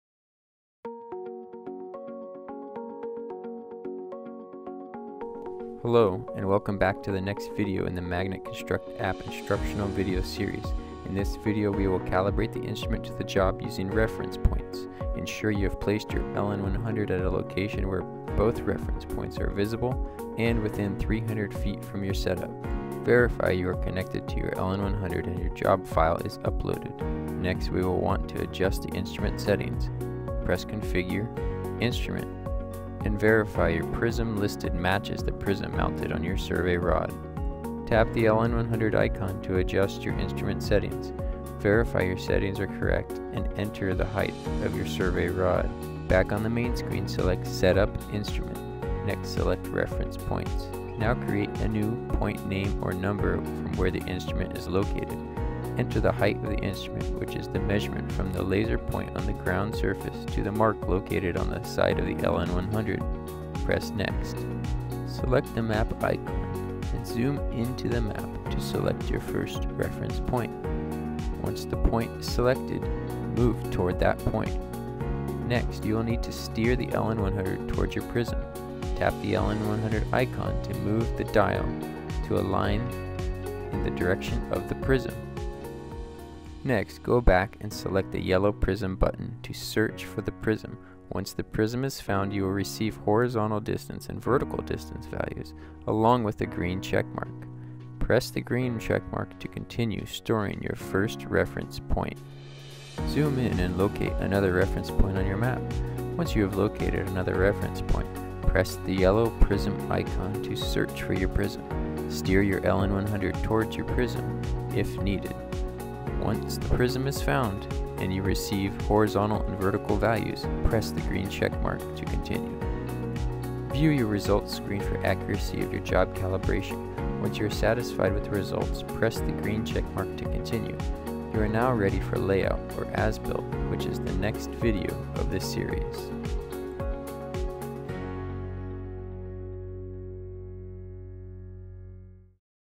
MAGNET Construct - Reference Point setup routine
MAGNET Construct offers multiple setup options for the Topcon LN-100 layout navigator.  This is in response to the reality that your jobsite circumstances change from project to project.
In this quick video, check out the fast and flexible option to physically setup the Topcon LN-100 layout navigator at a random location that is efficient and most safe for you.  Quickly measure two or more existing control points and the no-cost MAGNET Construct app will take it from there.